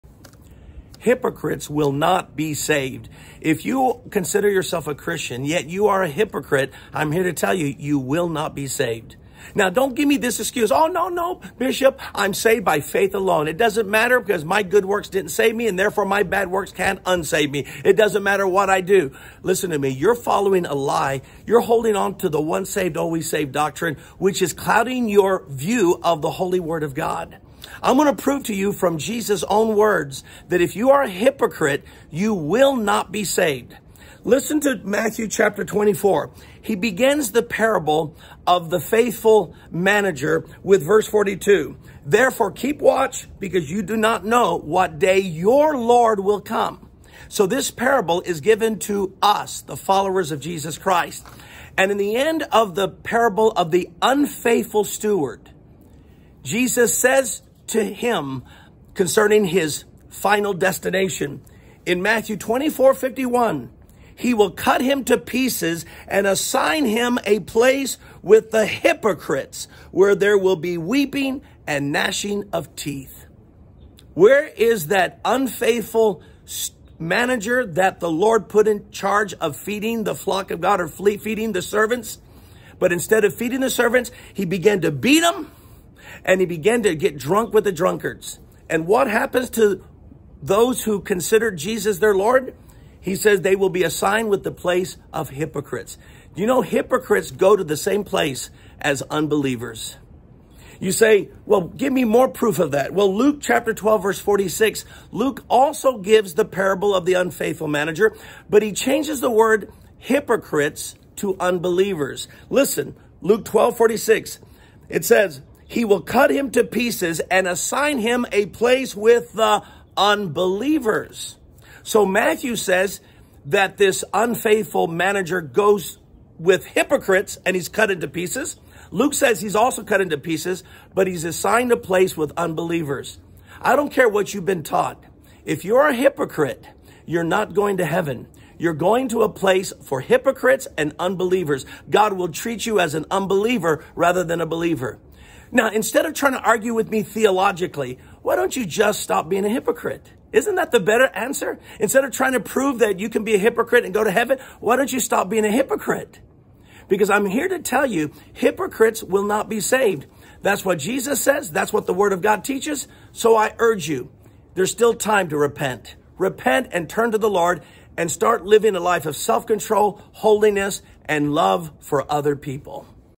Hypocrites Will Not Be Saved
Jesus parable about the unfaithful manager shows that what is paramount is not faith only but acting in holiness and love. Otherwise the believer ￼becomes a hypocrite and will be assign a place with unbelievers in the end. ￼